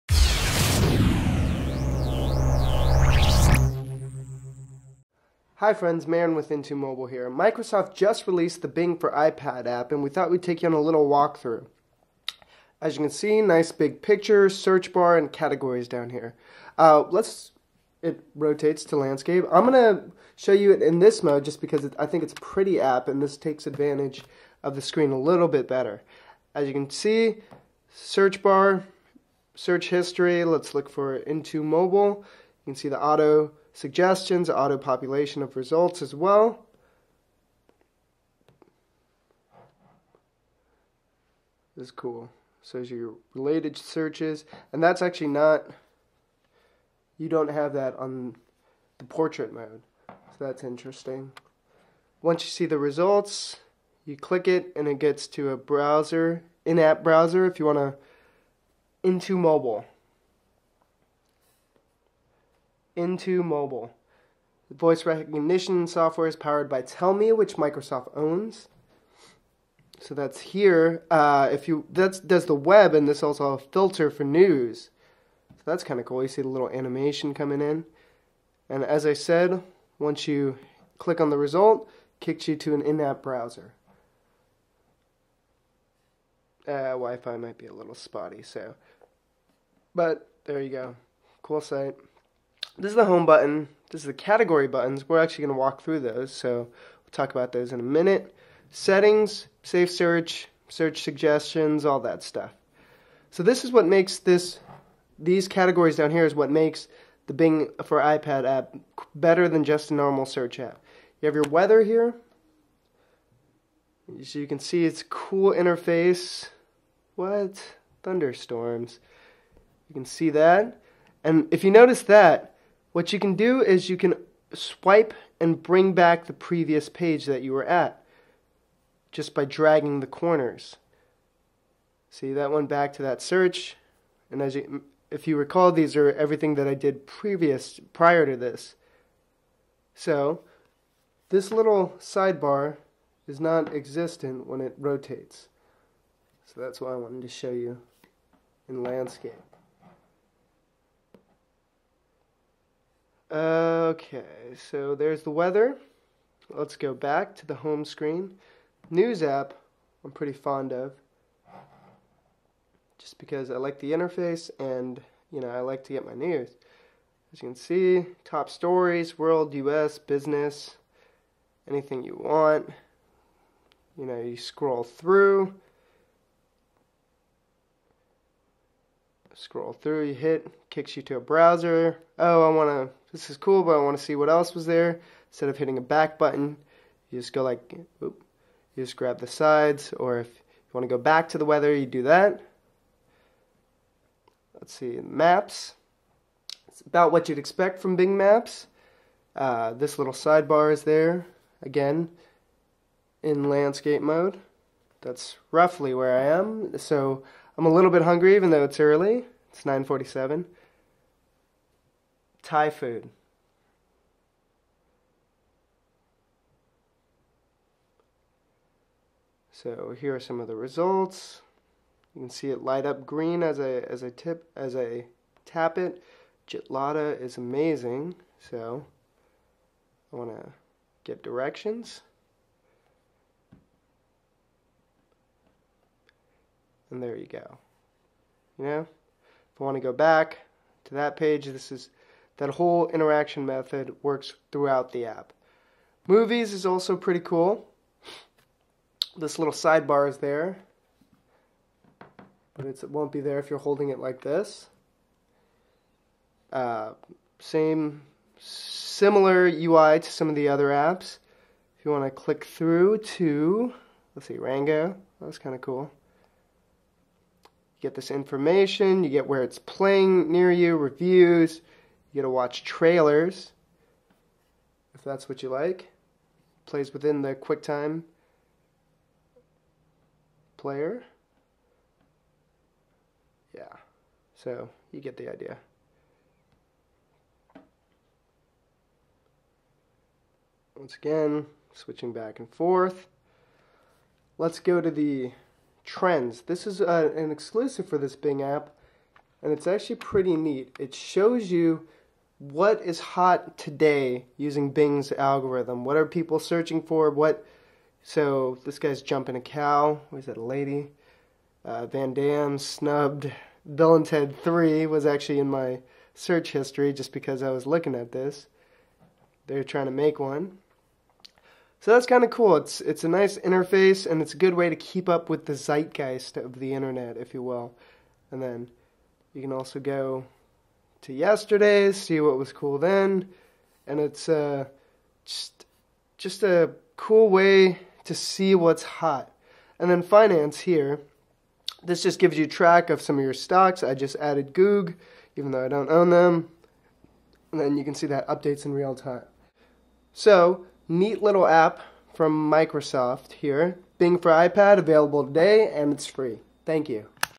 Bing iPad app demo: Voice search, Trends, Movies and more
We look at the Microsoft Bing iPad and it's actually pretty cool. It lets you search the web, browse through news, movies and trends in a visually-pleasing way that is optimized for the tablet.